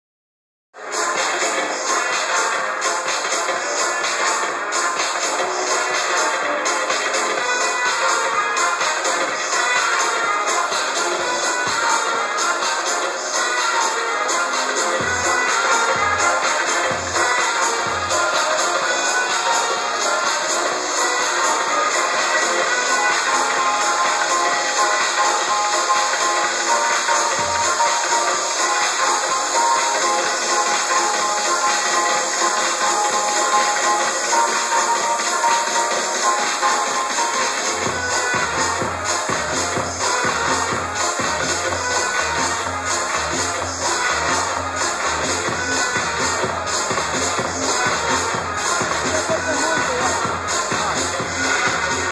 Apufest en Lima Norte - Dj Angelo Ortiz.
Cuando la electrónica se hacia presente en el bulevar de Los Olivos.